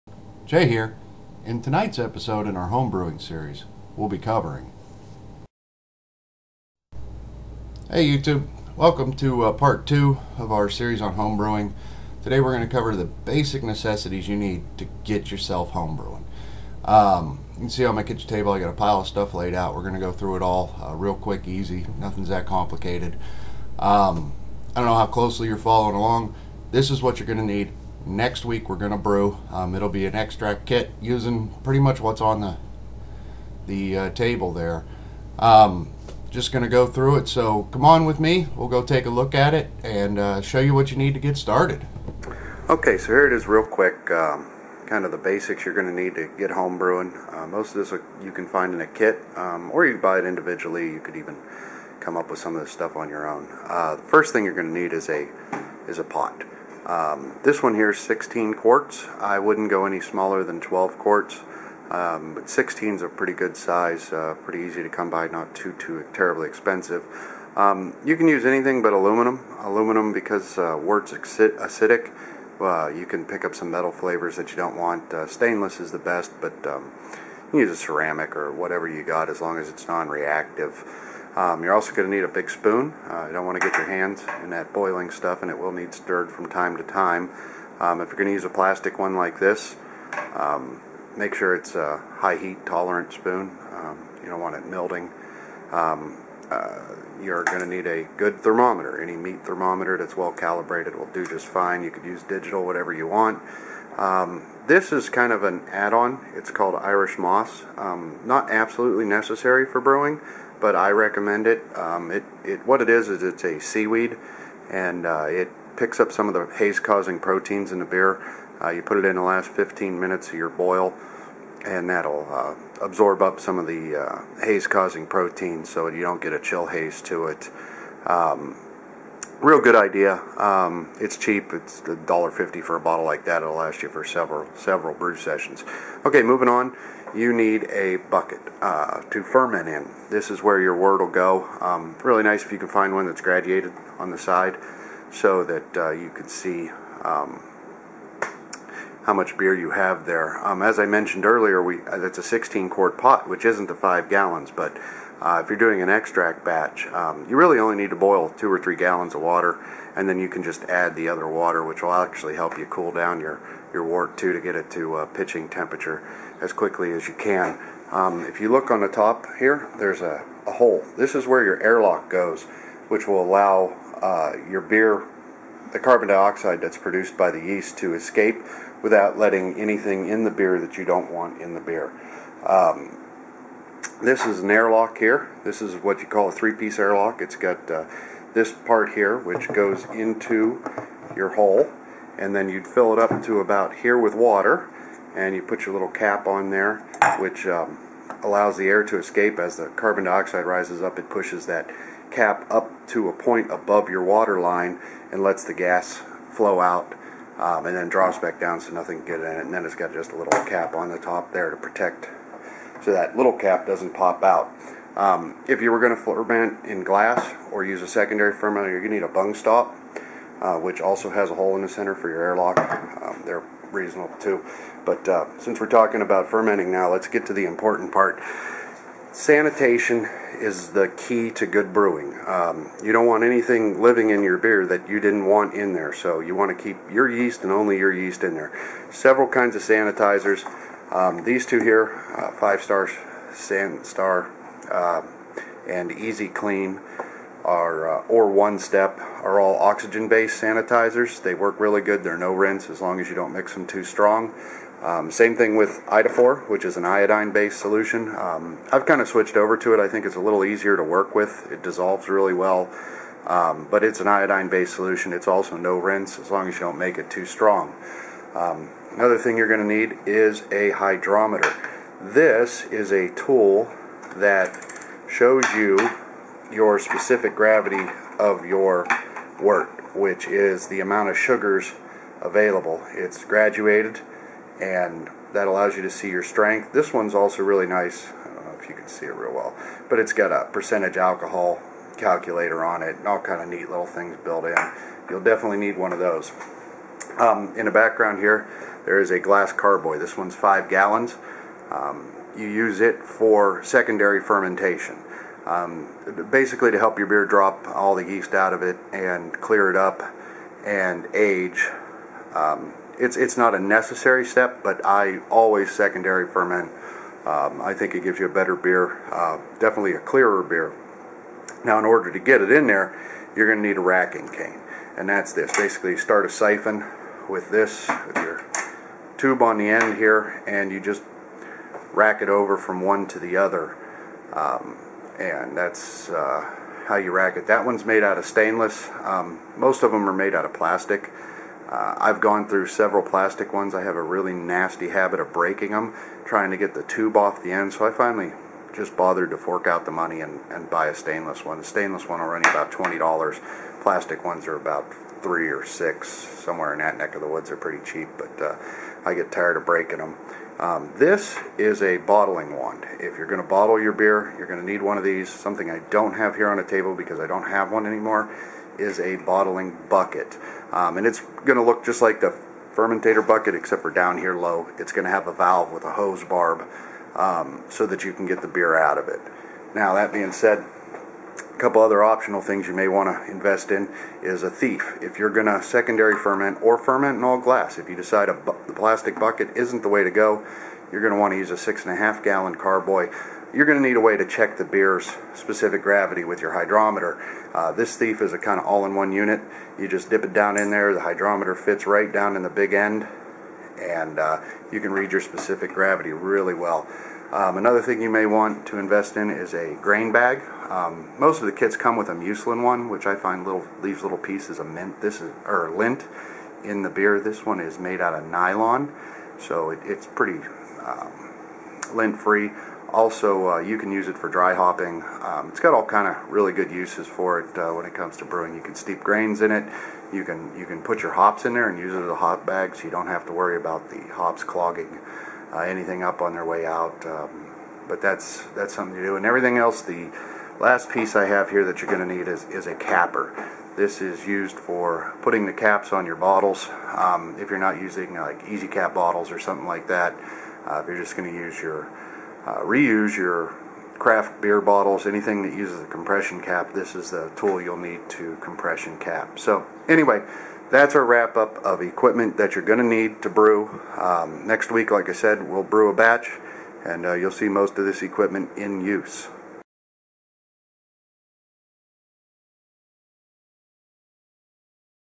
Homebrewing- basic equipment
In this episode of our homebrewing series i give an overview of the basic equipment you will need to brew.